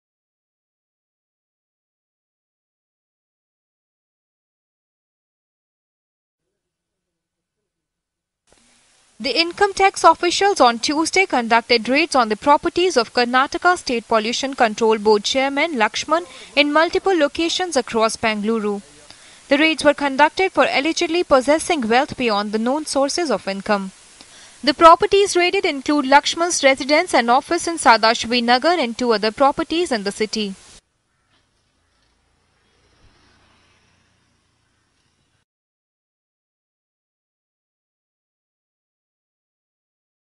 IT raids pollution board chief's residence
Bengaluru: Income Tax Department officials on Tuesday searched the residence here of Karnataka State Pollution Control Board Chairman Lakshman for alleged tax evasion, an official said.